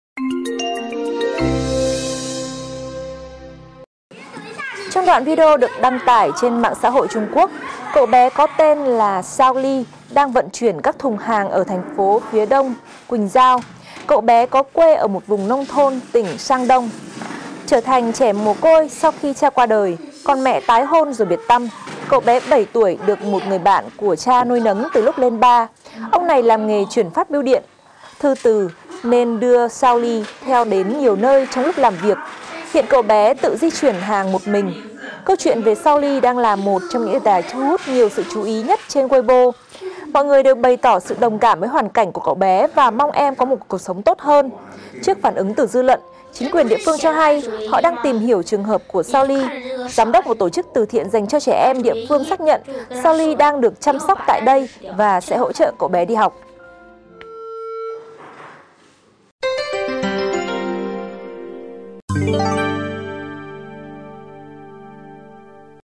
VTC Tin Mới
VTC1 | Câu chuyện về một bé trai 7 tuổi làm nghề chuyển hàng tiếp tục khơi dậy cuộc tranh luận về thực trạng đói nghèo của trẻ em Trung Quốc.

Xem bản full tại đây: goo.gl/Cjq8Lw